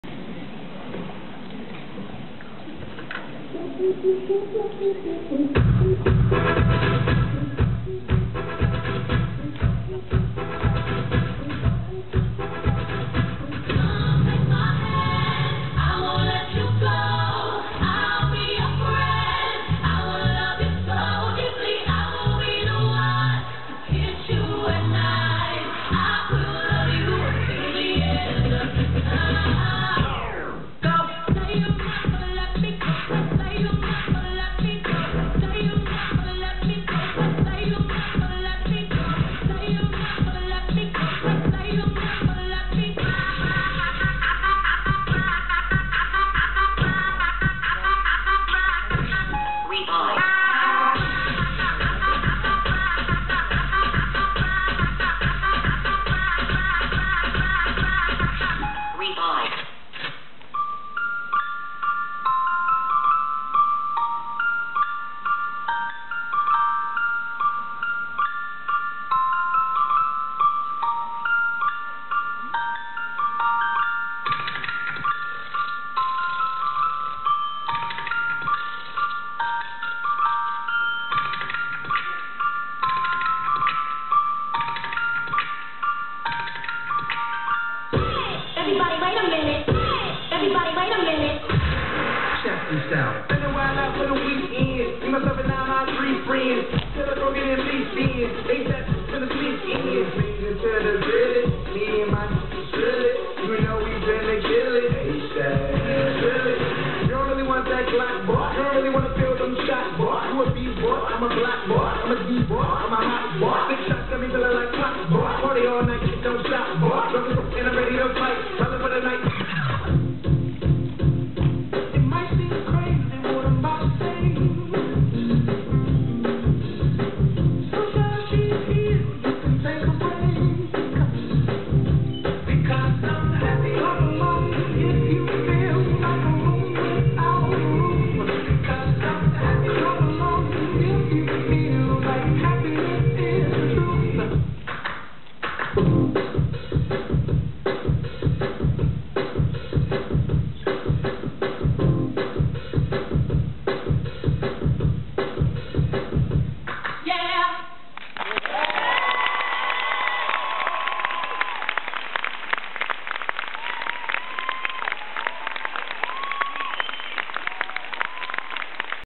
ShowTime Concours international de Biarritz
Concours international de Biarritz
1er Mai 2014
Médaille d'Or Groupe Catégorie 2